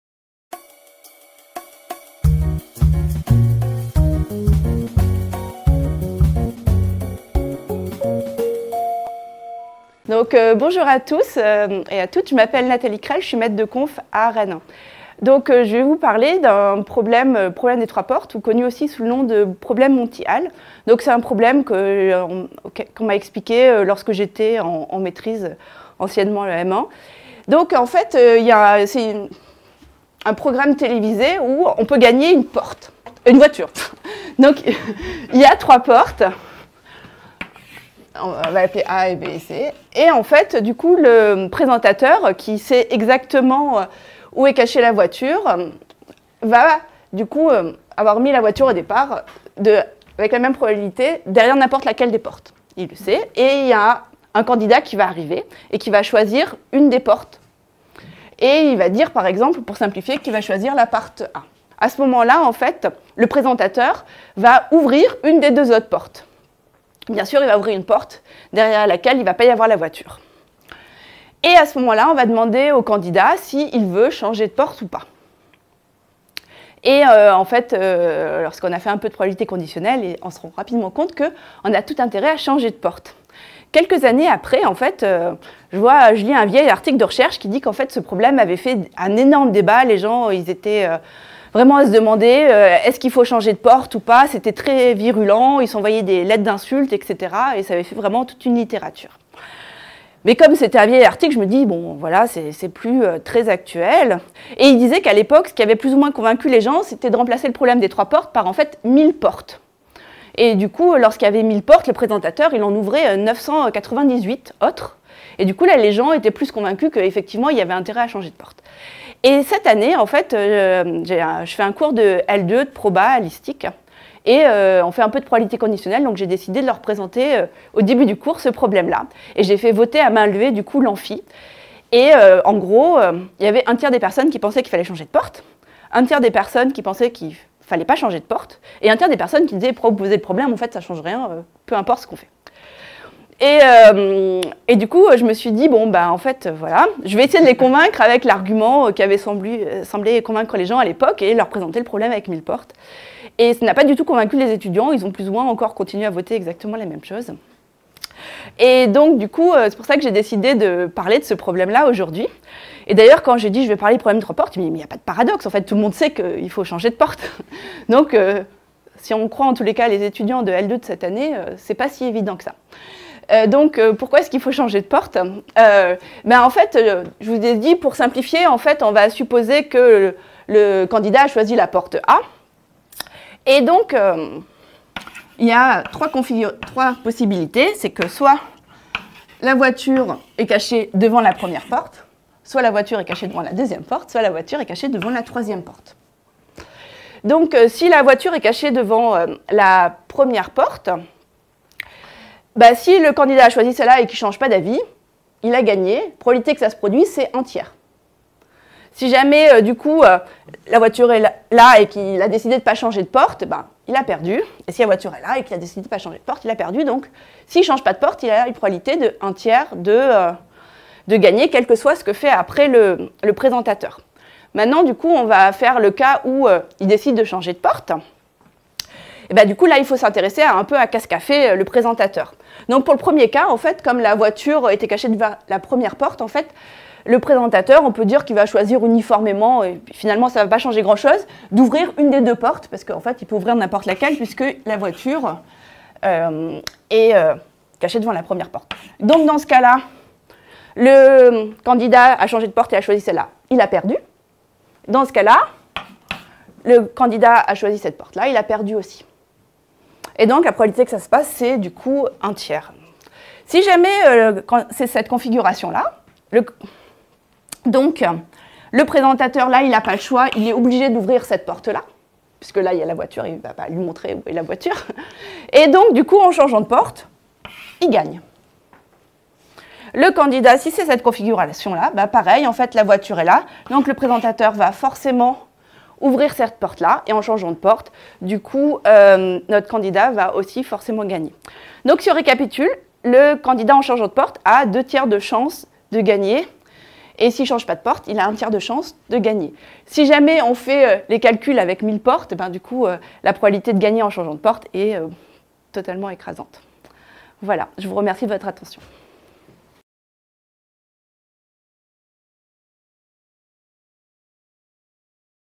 Nathalie Krell - Problème de Monty Hall, un vrai paradoxe ?
Un joueur est placé devant trois portes fermées. Derrière l'une d'elles se trouve une voiture et derrière chacune des deux autres se trouve une chèvre. Le joueur doit choisir une porte. Puis le présentateur doit ouvrir une porte qui n'est ni celle choisie par le candidat, ni celle cachant la voiture (le présentateur sait quelle est la bonne porte dès le début). Le candidat a alors le choix entre ouvrir la porte qu'il a choisie initialement, ou bien ouvrir la troisième porte. Laquelle doit-il choisir, s'il veut maximiser ses chances de voir la voiture derrière la porte qu'il a ouverte?